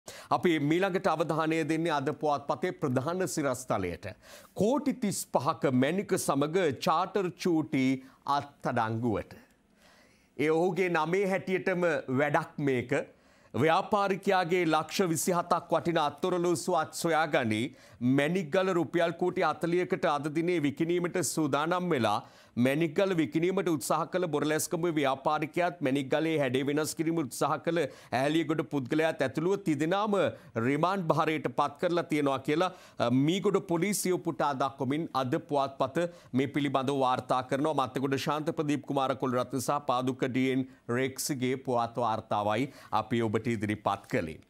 කෝටි 35ක මැණික සමග "චාටර් චූටි" අත්අඩංගුවට
🔴 PATHTHARE ll පත්තරේ ll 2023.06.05